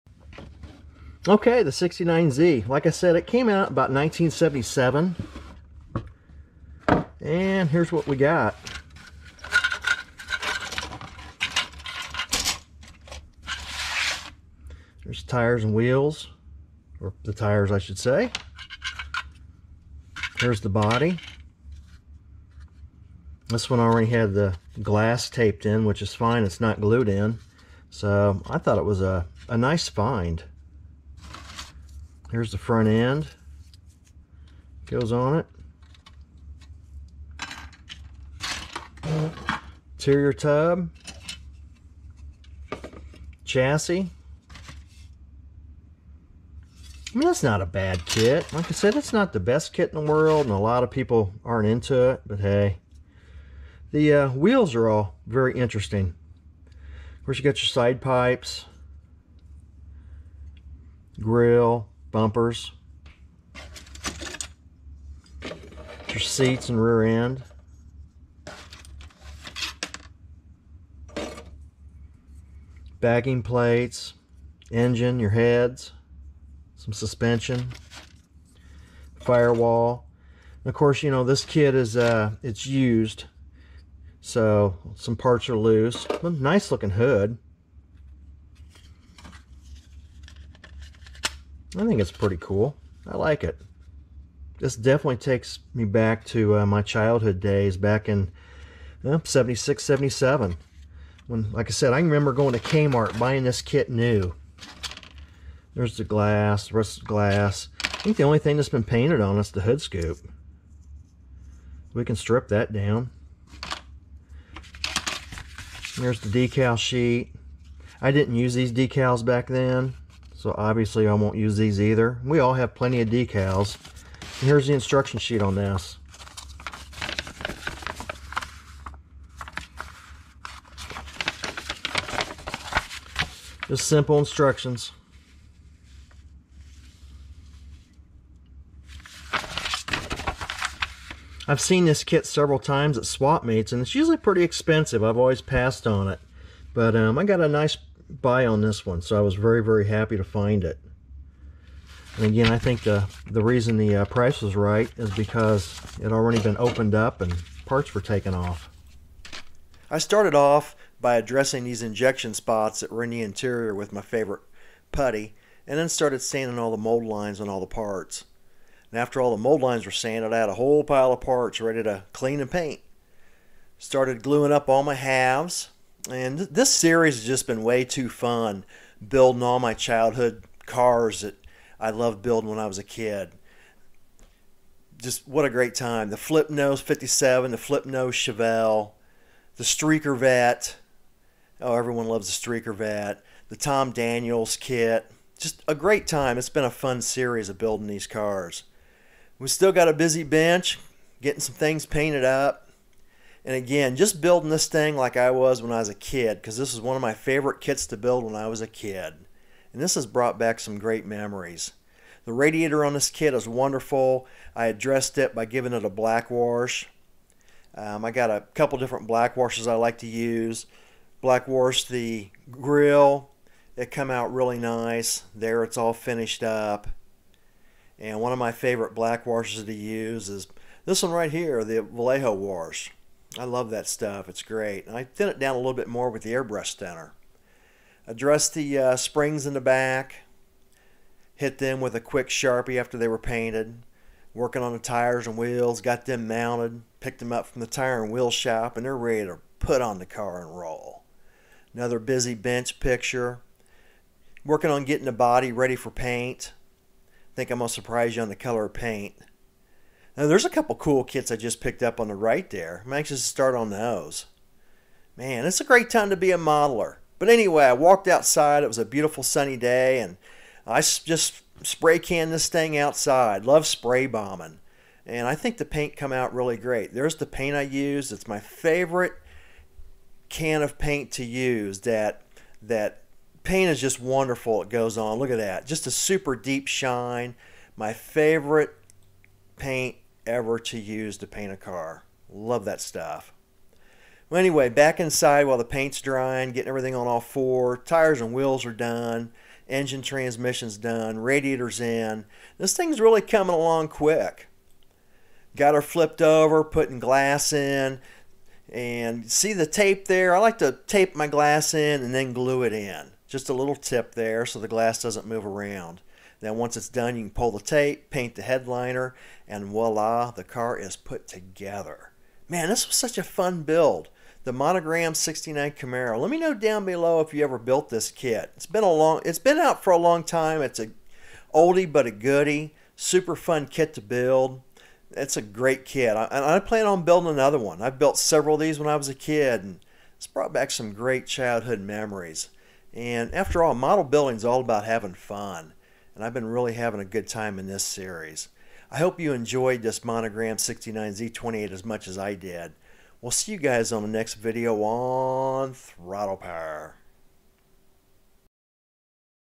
Monogram '69 Z28 Build Tips and Tricks!
Review and build the Monogram '69 Z28

Tips, Tricks How To and More!

Throttle Power